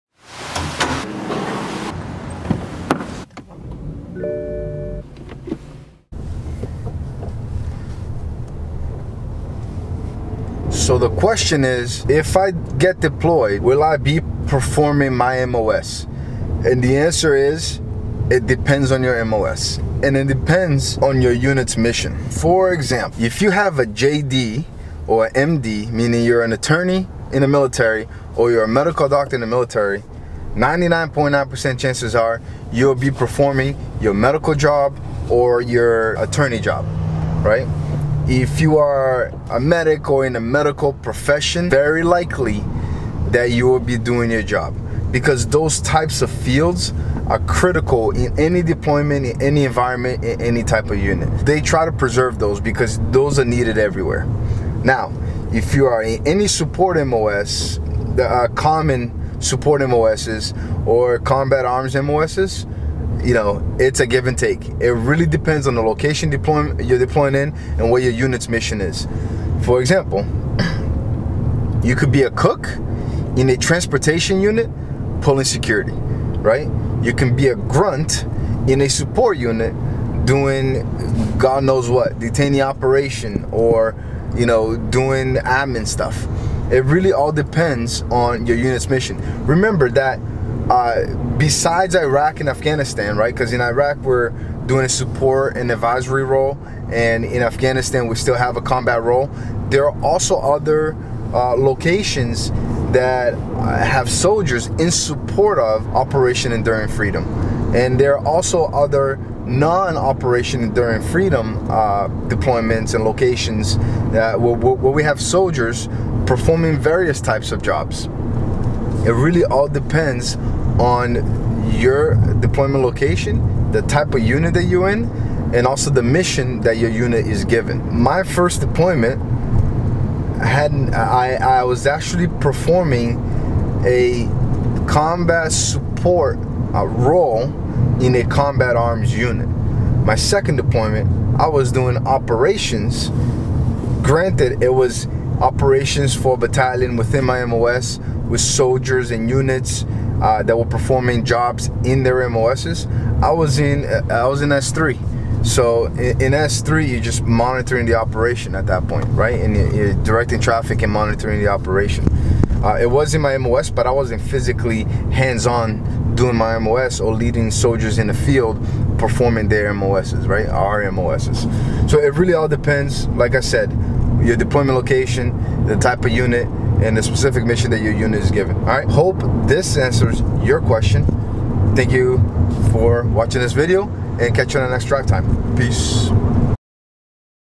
DT-02 Deployment MOS - Army National Guard
In this video, I discuss if you will be doing your MOS during a deployment.

Alex